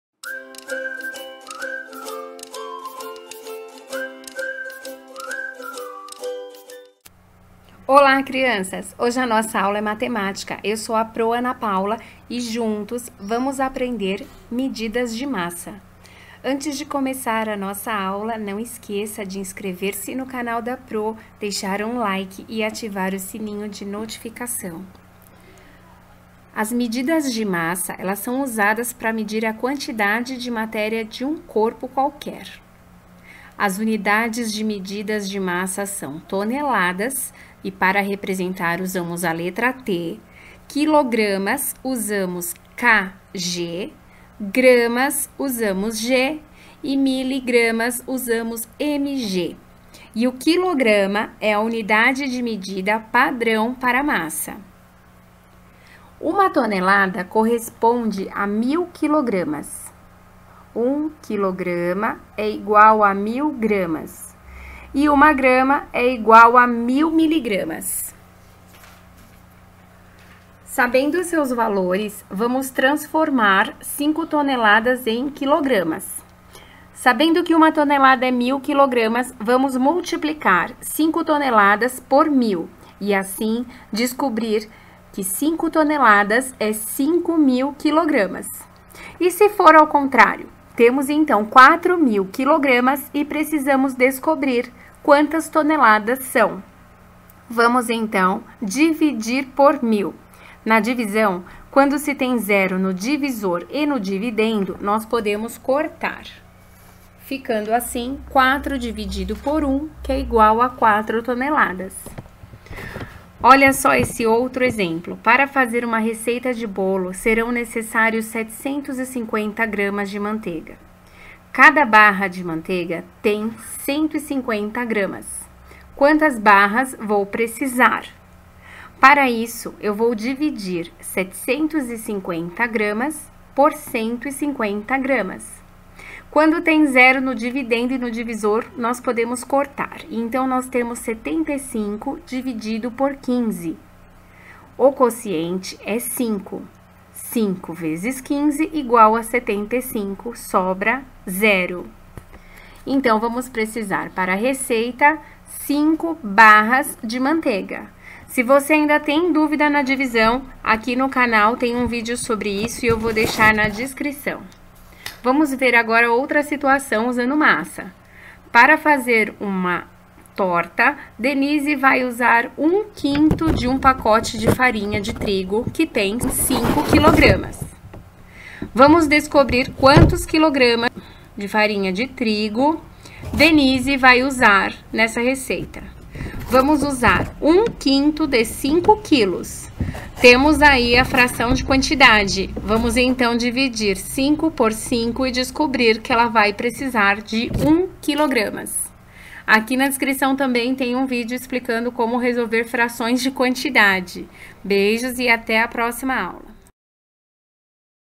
PRINCIPAIS MEDIDAS DE MASSA (TONELADAS, QUILOGRAMAS, GRAMAS E MILIGRAMAS)
CONVERSÃO DE UNIDADE DE MASSA E SITUAÇÃO PROBLEMA ENVOLVENDO MEDIDAS DE MASSA